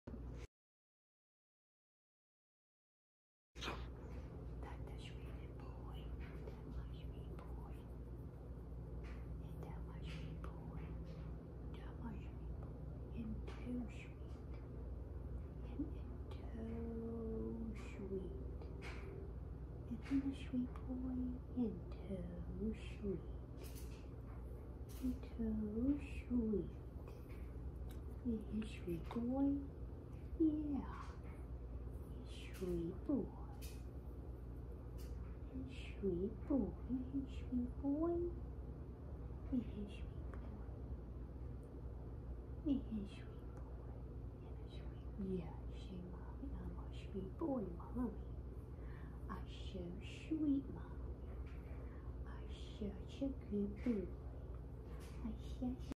PitBull Dog Massage Therapy 101.
Think the world just needs a sweet moment right now. Please enjoy, share and spread kindness today🐾
Arch the PitBull pup

More Arch on TikTok:
archugladimhere